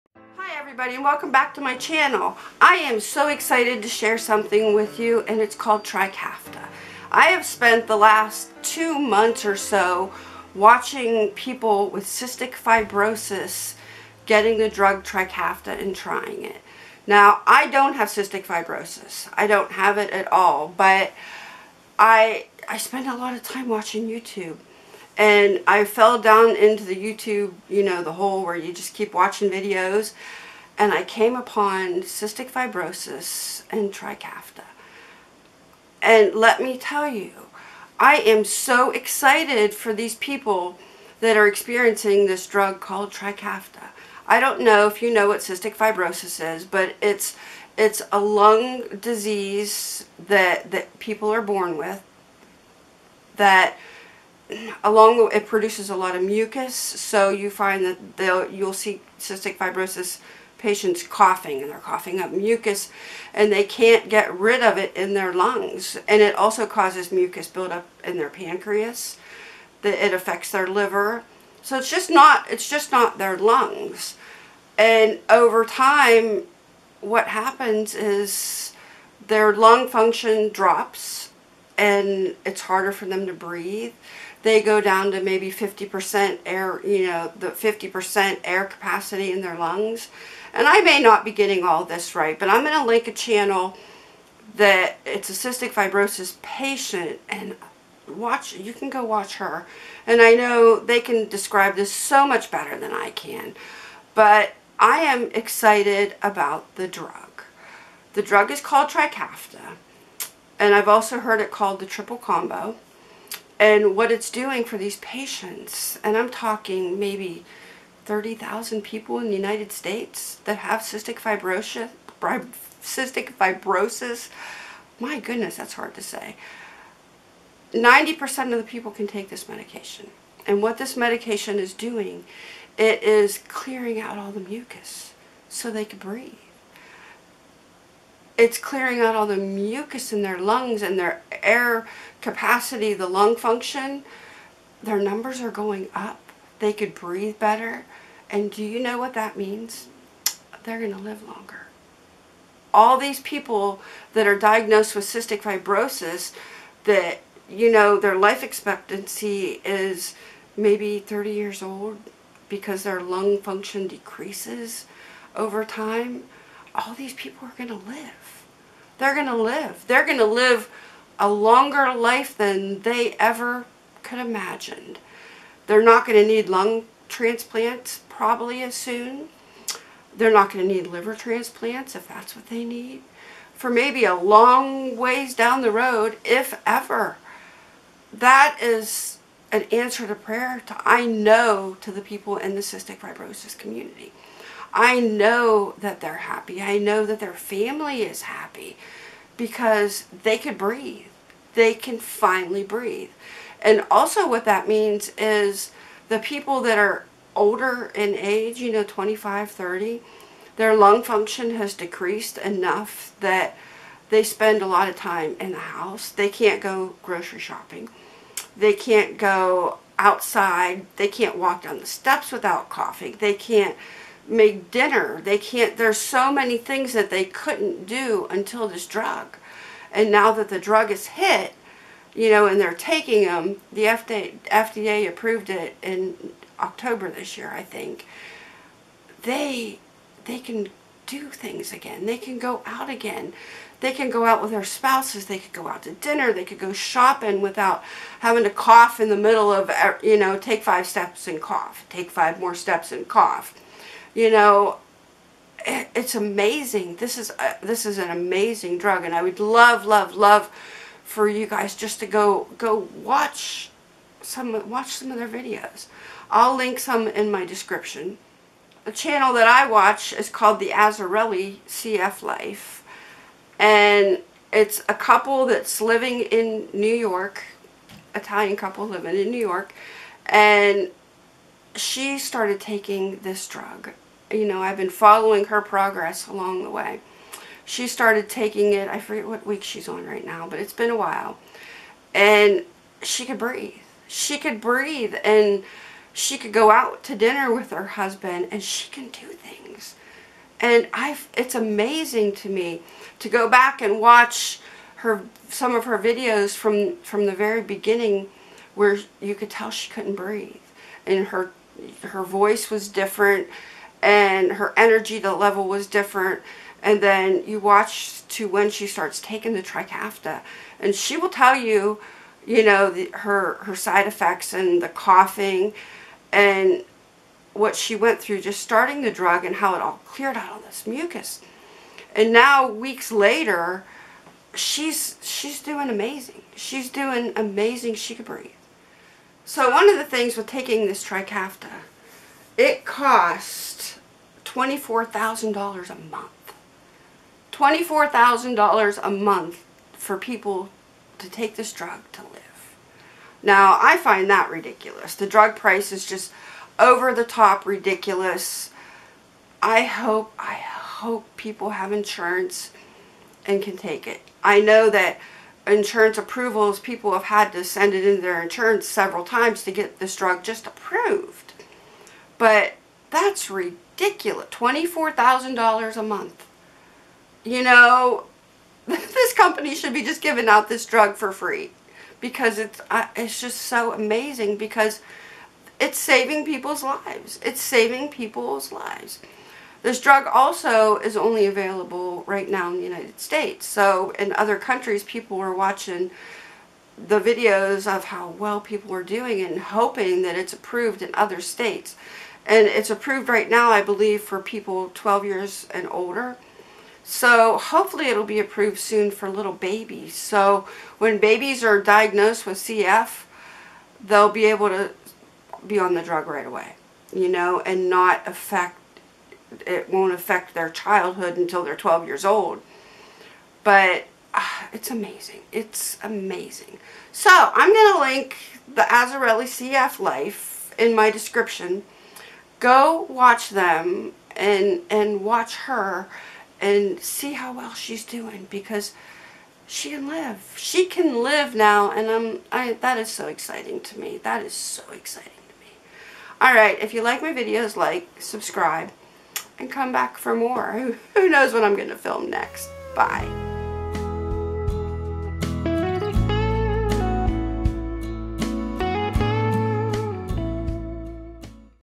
Cystic Fibrosis and Trikafta
The FDA Has approved a new drug called Trikafta for Cystic Fibrosis (CF) patients.  Trikafta is a ground breaking miracle drug that allows patients with cystic fibrosis to breathe better and live longer.  Praise God!!!  I am so happy for the people with cf that are able to take this medication!

***THE AZZARELLI CF LIFE...